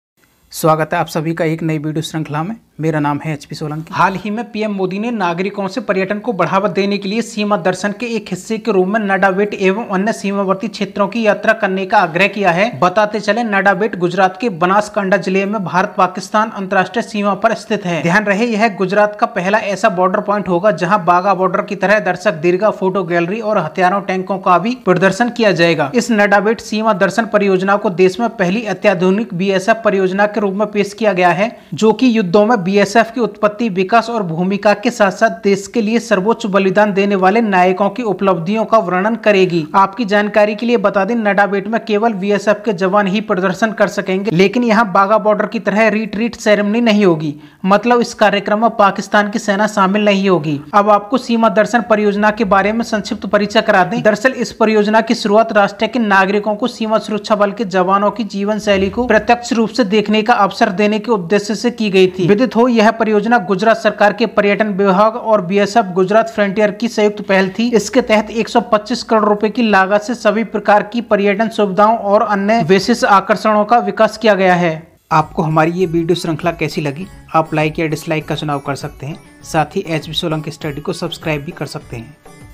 चर्चा में नडाबेट सीमा दर्शन परियोजना क्या है? और इसका संबंध किस राज्य से है! nadavet Seema Darshan
चर्चा में नडाबेट सीमा दर्शन परियोजना क्या है? और इसका संबंध किस राज्य से है!
 nadavet Seema Darshan